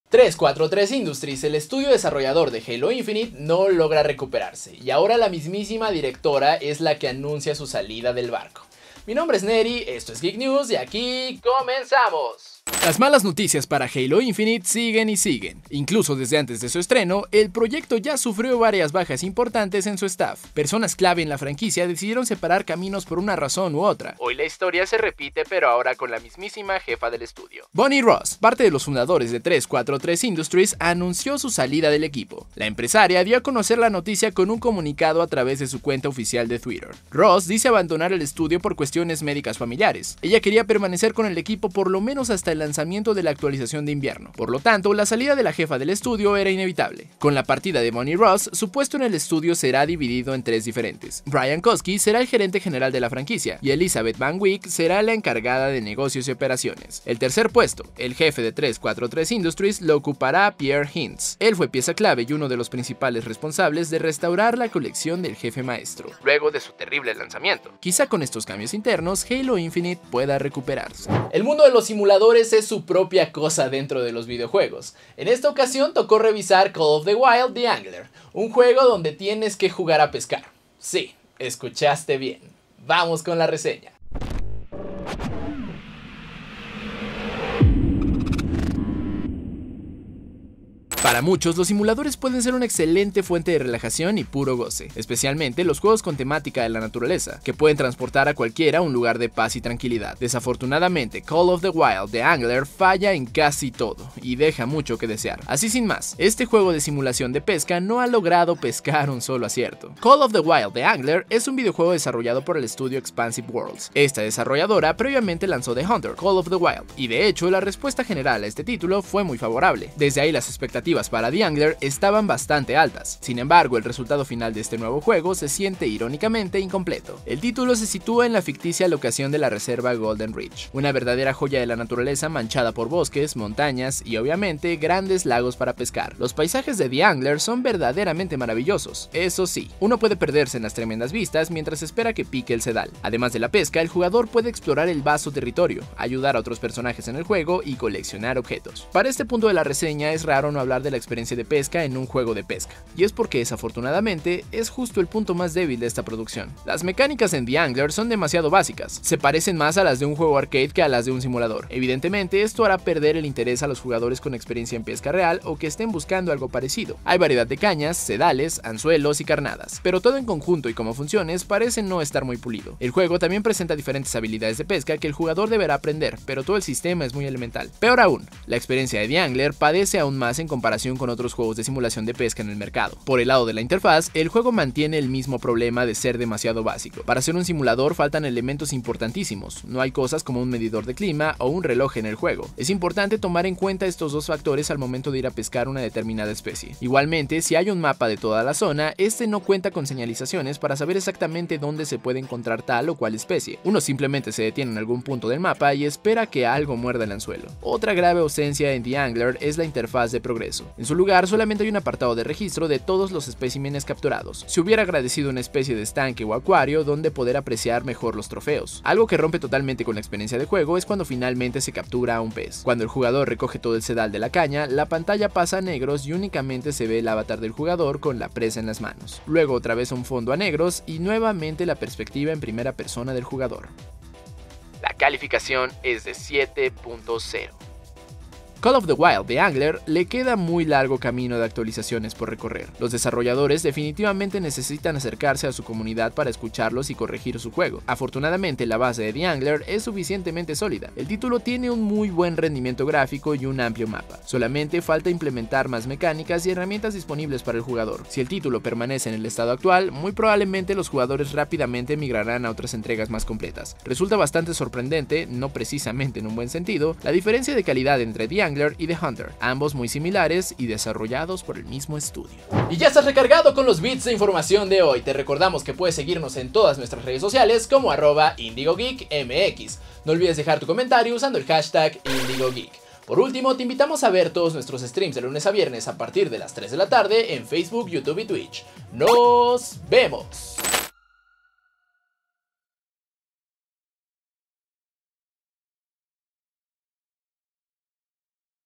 Bonnie Ross abandona 343 Industries y Halo Infinite
¡Hoy en GeekNews de IndigoGEEK!

00:00 Geek News 👾
00:12 Bonnie Ross abandona 343 Industries
02:30 REVIEW Call of the Wild: The Angler
04:32 Despedida 👋

-La directora de 343 Industries abandona la compañí
-Y te contamos si Call of the Wild: The Angler vale la pena